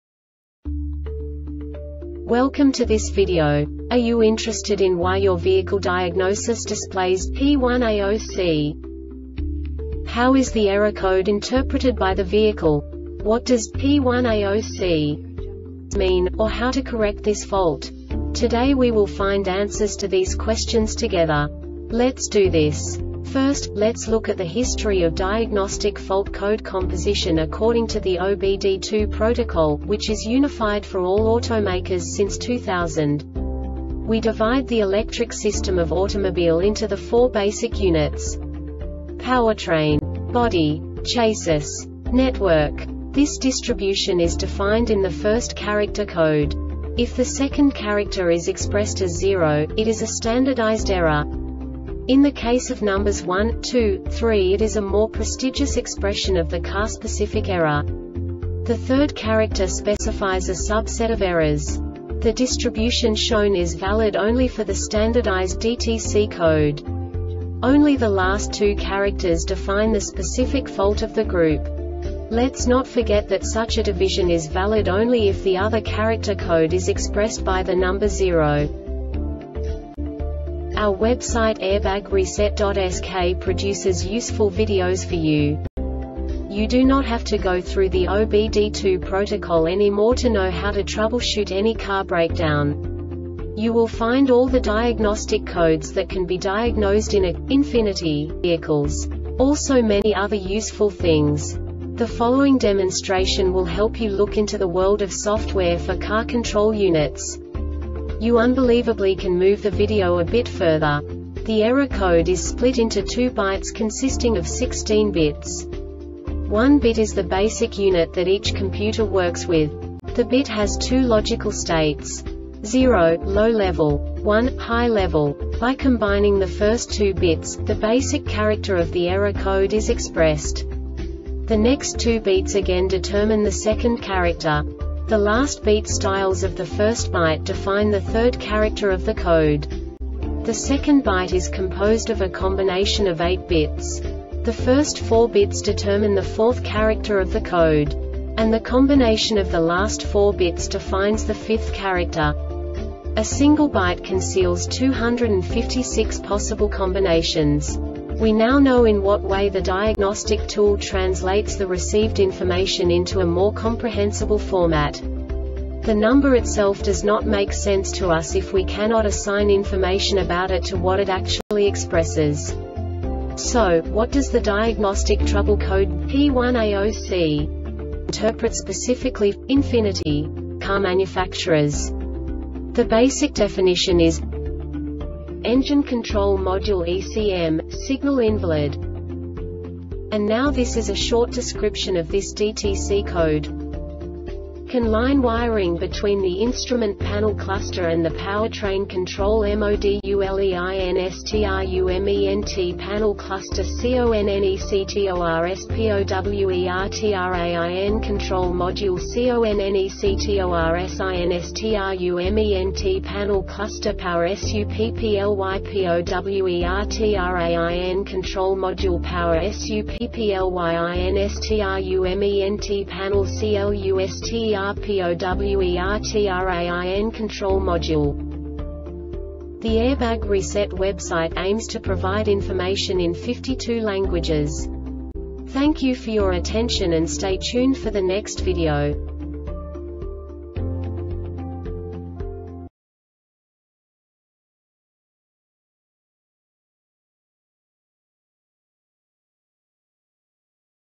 DTC Ford P1A0C Short Explanation
Contents:
0:21 Basic DTC analysis according to OBD2 protocol standard.
1:48 Insight into programming
2:58 A diagnostically understandable description of the Diagnostic Trouble Code
3:05 Basic error definition

Make:
Ford
Code:
P1A0C
Definition:
Engine Control Module (ECM) - Signal Invalid
Cause:
CAN LINE WIRING BETWEEN THE INSTRUMENT PANEL CLUSTER AND THE POWERTRAIN CONTROL MODULEINSTRUMENT PANEL CLUSTER CONNECTORSPOWERTRAIN CONTROL MODULE CONNECTORSINSTRUMENT PANEL CLUSTER POWER SUPPLYPOWERTRAIN CONTROL MODULE POWER SUPPLYINSTRUMENT PANEL CLUSTERPOWERTRAIN CONTROL MODULE